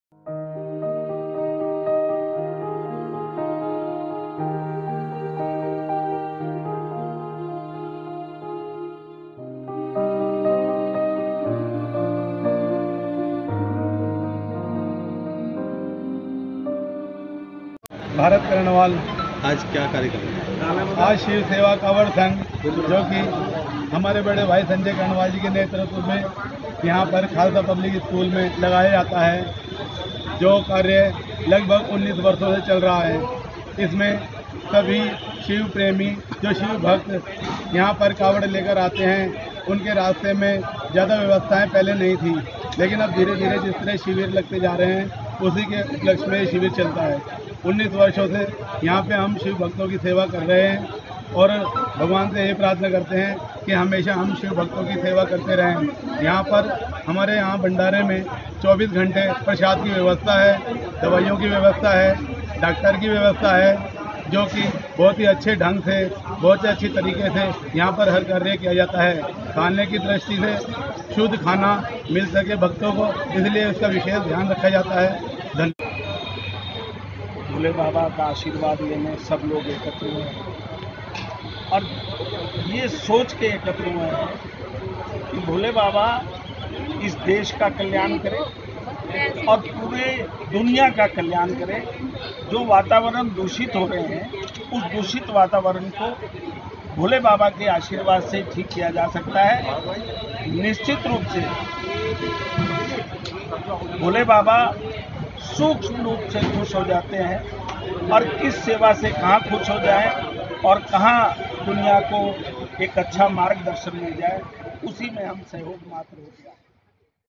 कावड़ शिविर का शुभारंभ, रिपोर्ट खेमचंद सैनी
शिव कावड़ सेवा संघ का पूजा अर्चना के साथ शुभारंभ
शिव कावड़ सेवा संघ का देहरादून रोड पर खालसा पब्लिक स्कूल में आज पूजा अर्चना के साथ आज गणमान्य लोगों की उपस्थिति में विधिवत शुभारंभ हो गया ।।इस दौरान भारत कर्णवाल, संजय कर्णवाल, नीरज गुप्ता, कांग्रेस नेता महेंद्र तनेजा के अलावा अनेको गणमान्य लोग उपस्थित रहे।।